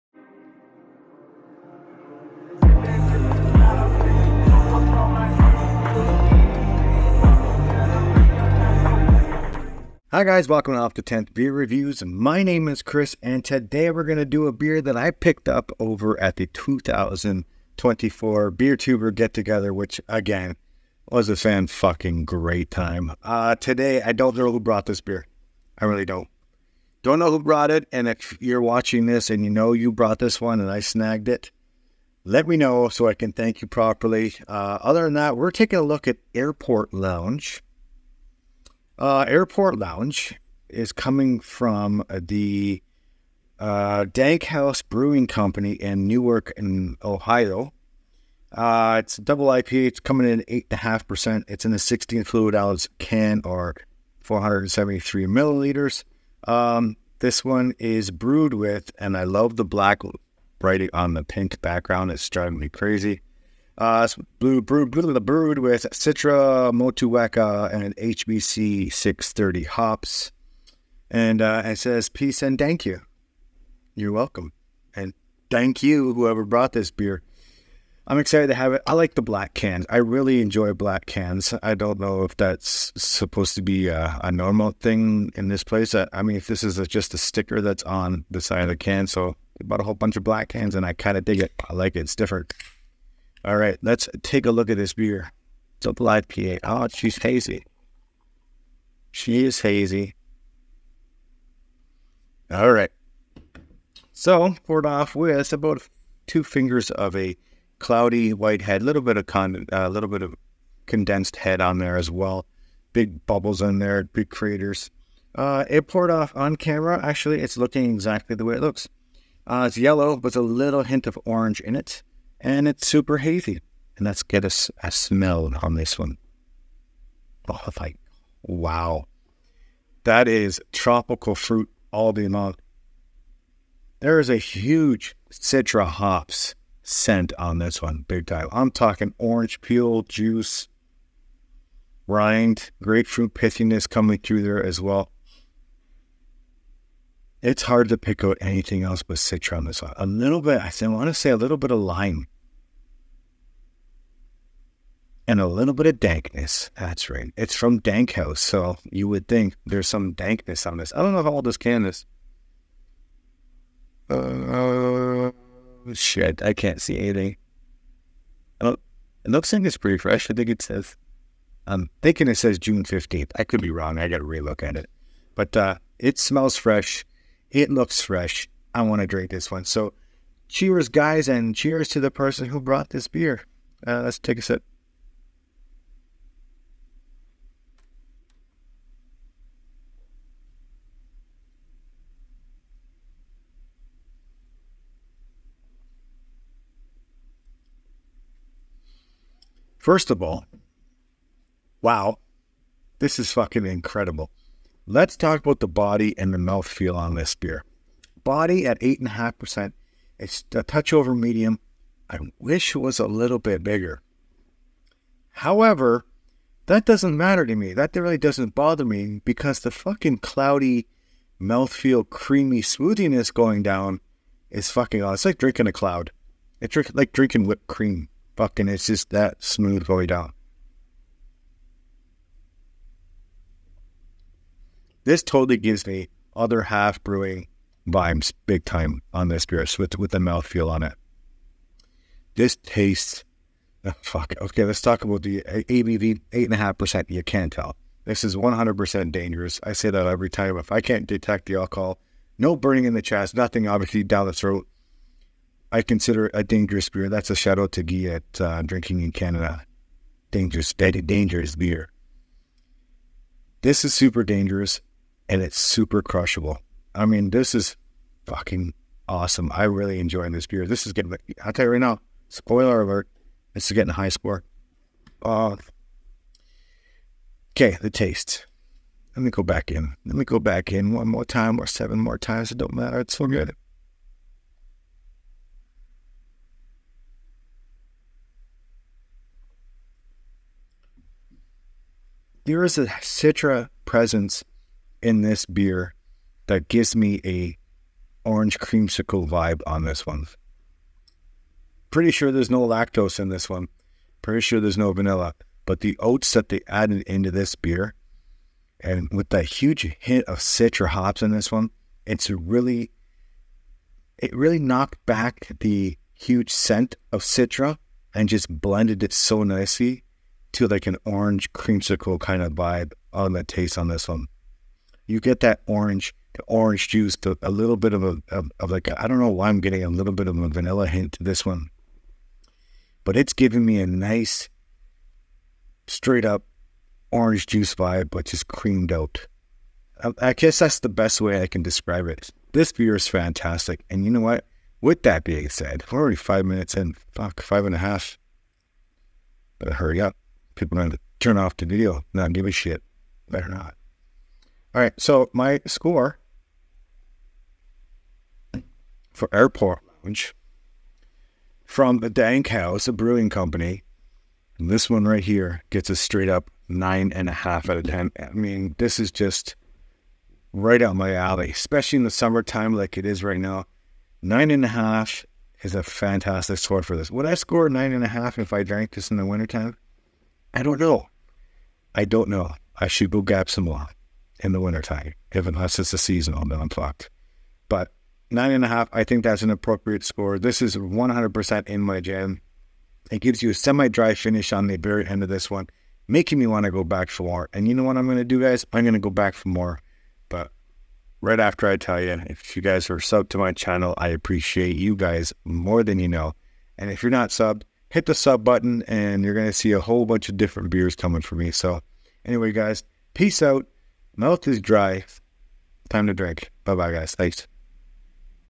Airport Lounge DIPA - Dankhouse Brewing Co. - 8.5%
Today, Off The 10th, I will give you My Own Opinion. I will describe what I see, smell, and taste in this beer.